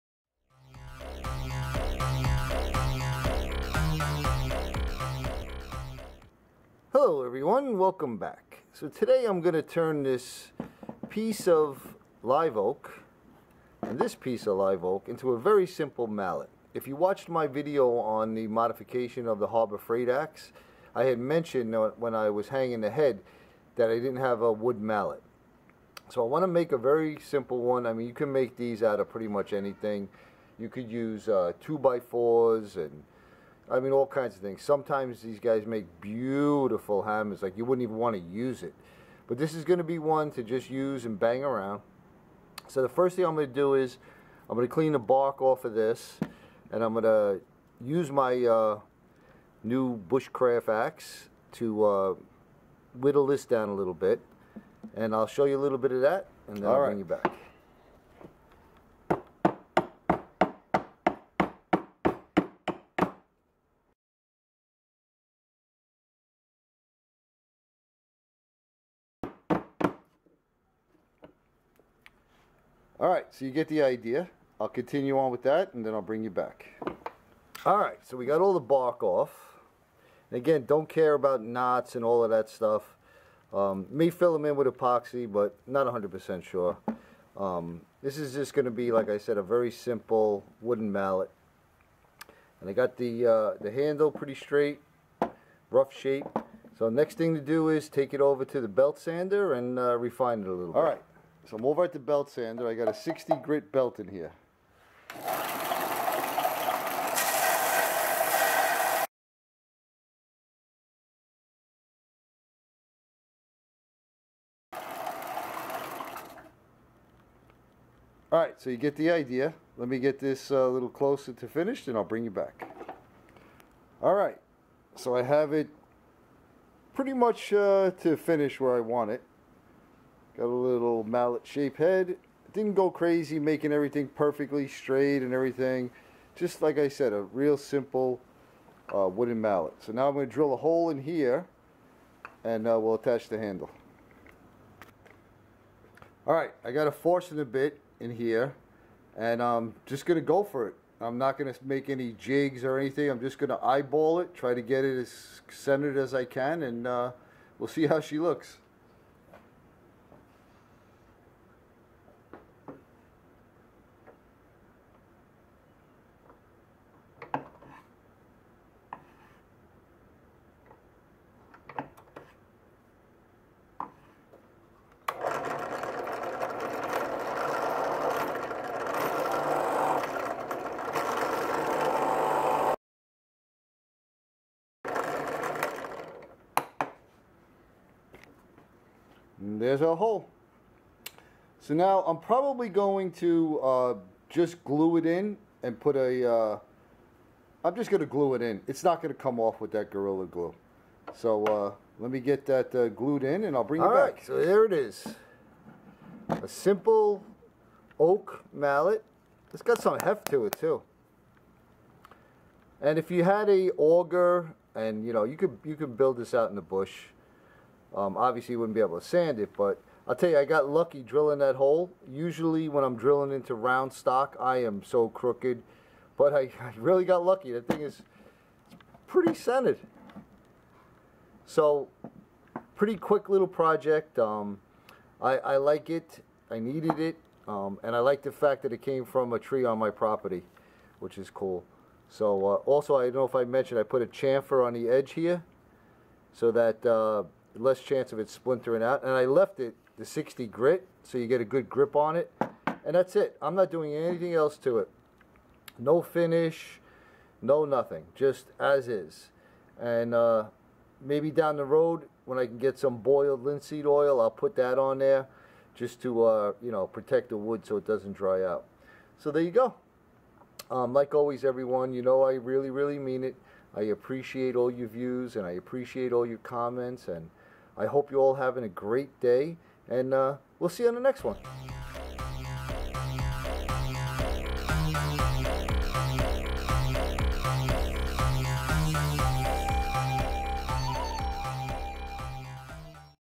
How to make a simple wooden mallet!
In this video I will show you how to make a simple wooden mallet!